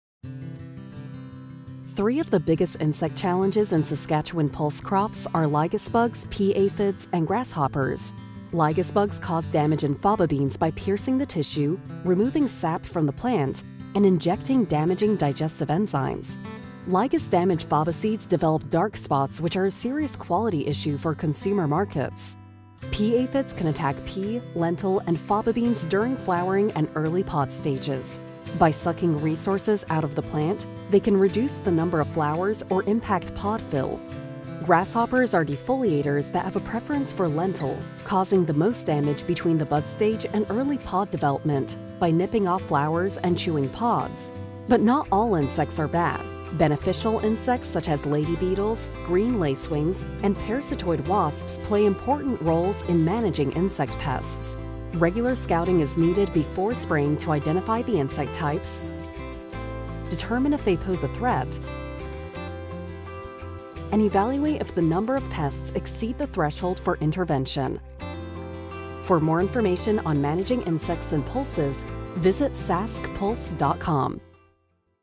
Managing Insects in Pulse Crops
Three of the biggest insect challenges in Saskatchewan pulse crops are lygus bugs, pea aphids, and grasshoppers - but not all insects are problematic. Watch our latest video and read the articles to learn about how to effectively manage insects in your pulse crops.